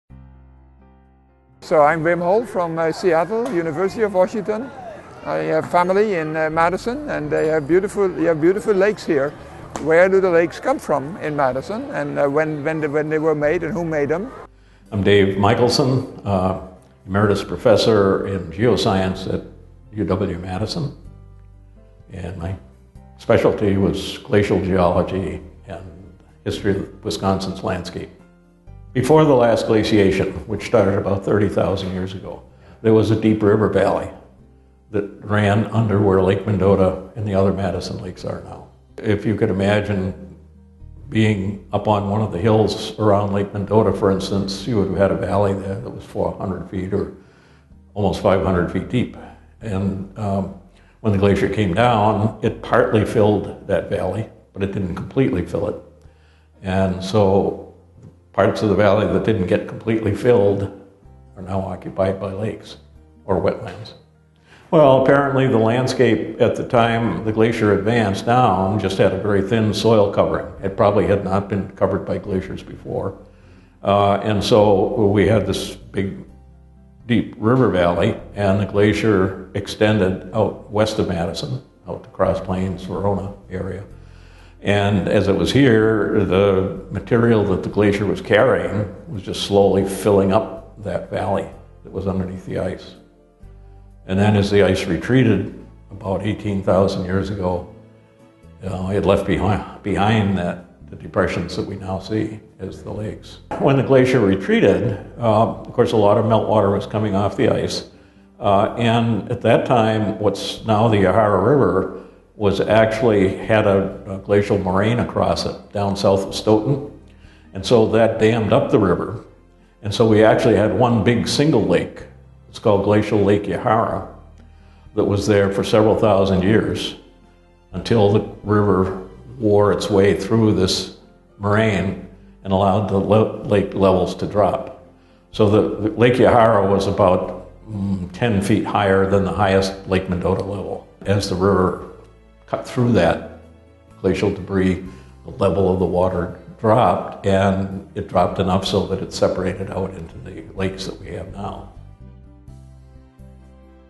Blue Sky Science: How were the Madison lakes formed?
Before the last glaciation, which started about 30,000 years ago, there was a deep river valley that ran under where Lake Mendota and the other Madison lakes are now. A glacier moved into this area and slowly filled up the valley.